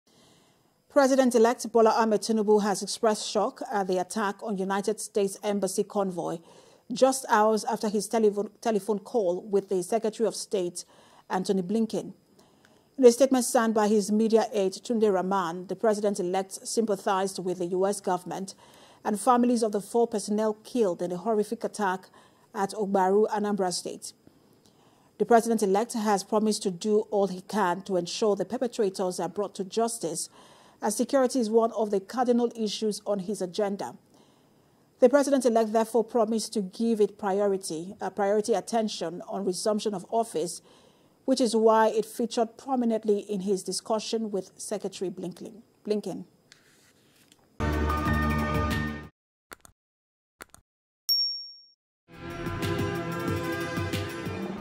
President-Elect Tinubu Sympathises With U.S., Condemns Attack On Embassy Staff
President-elect Bola Tinubu has expressed shock at the attack on United States Embassy convoy just hours after his telephone call with the Secretary of State Antony Blinken.
In a statement signed by his Media Aide, Tunde Rahman, the President-elect sympathized with the U.S Government and the families of the four personnel killed in the horrific attack at Ogabru, Anambra State.